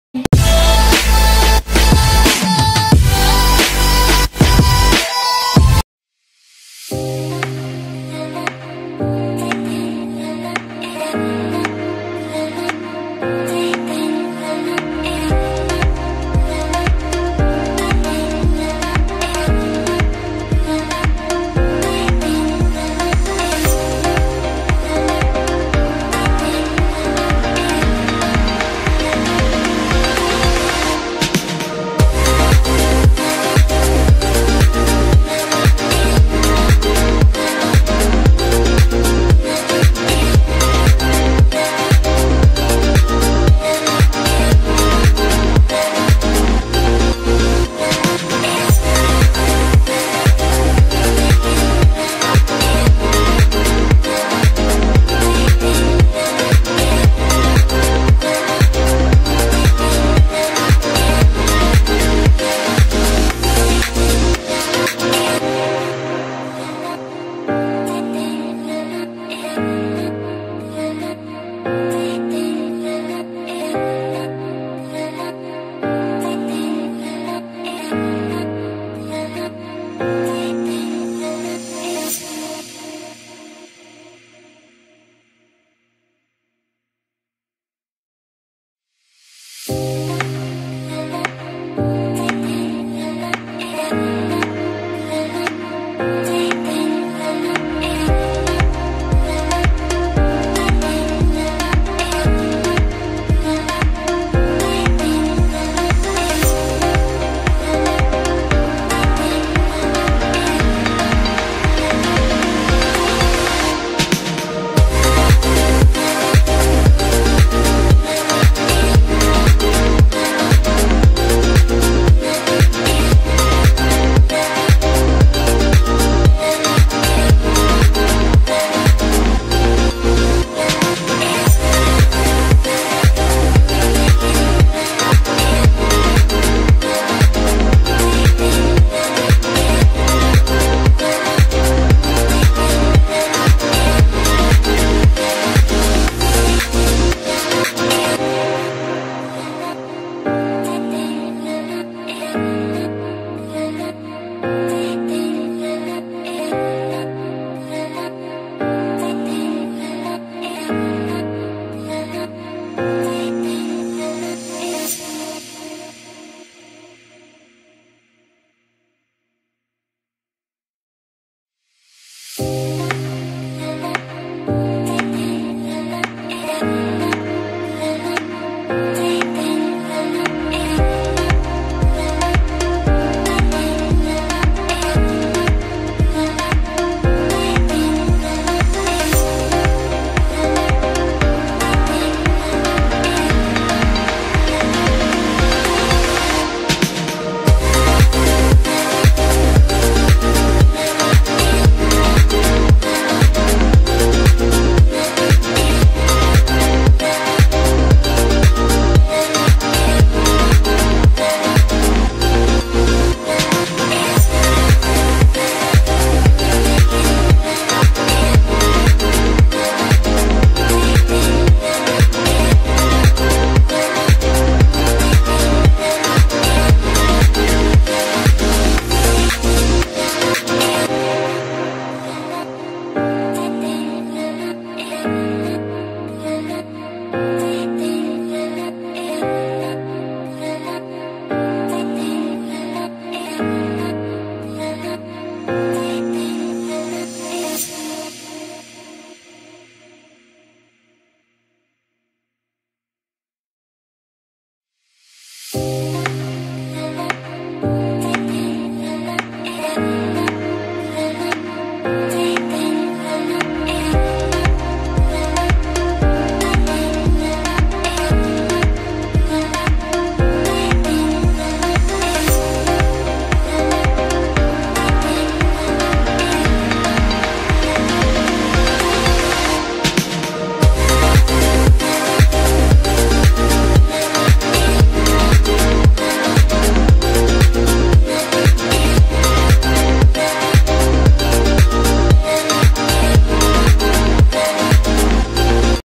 how to record zoom meeting with internal audio/external voice without host permission only for pcs😀
this is web base screen recorder so you have to pin to taskbar and create a shortcut so if you do that than click start screen recording from the web

for other video also

if any problems the message on :
instagaram= vaidehipatel635

it will 100% safe and it will help you for online classes 😀

hope it's helps you!

NOTE: pin the app to task bar if you don't than you will have to reinstall again so take a note on it